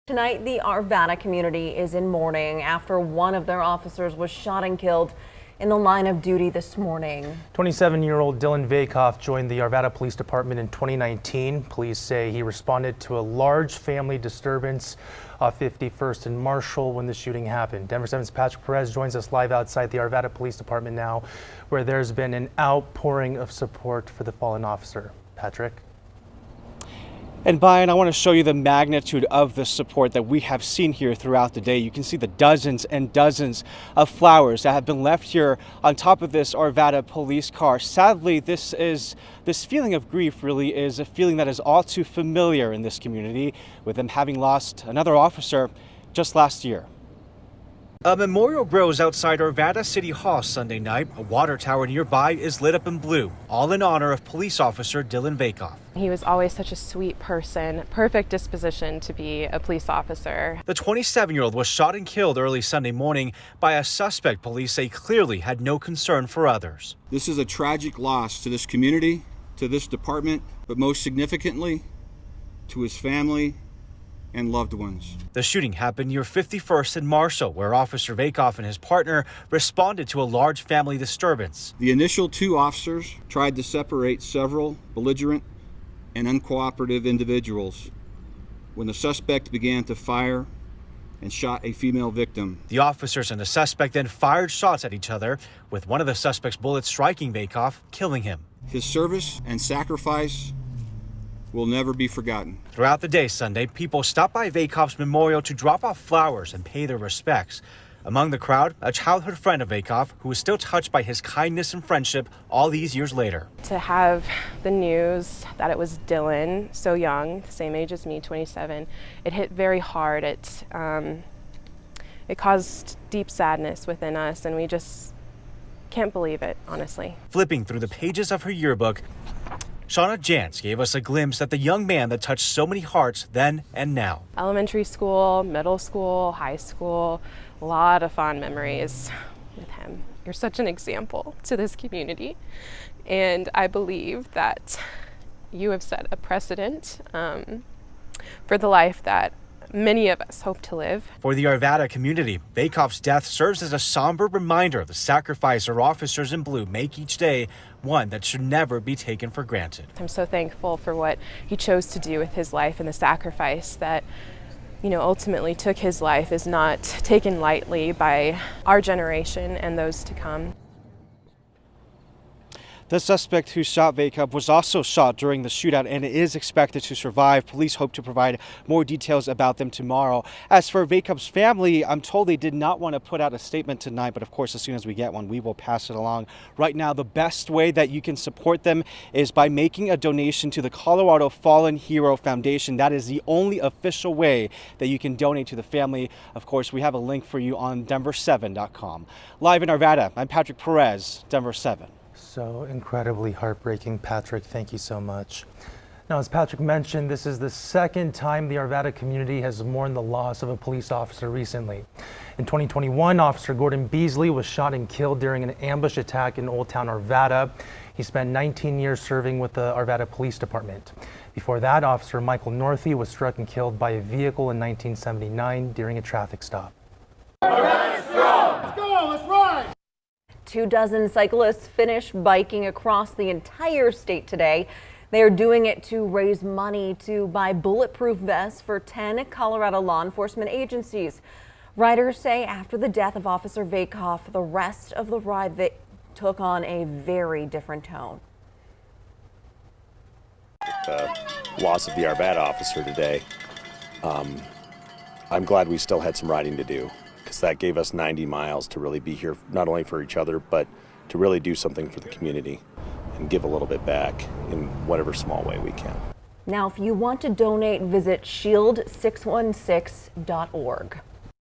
Arvada community mourns loss of Officer Dillon Vakoff
The Arvada Police Department is mourning the loss of one of its officers. Dillon Vakoff, 27, was shot and killed in the line of duty early Sunday morning.